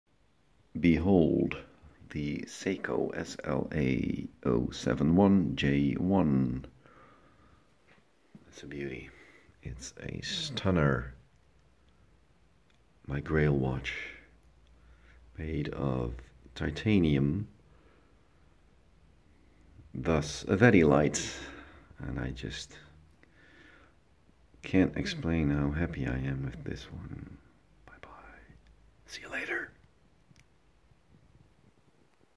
Seiko SLA 071J1 - My Grail Watch
Done with collecting since I got this wonderful beauty. This is it. This is where it all comes together.

Have you already found your grail watch?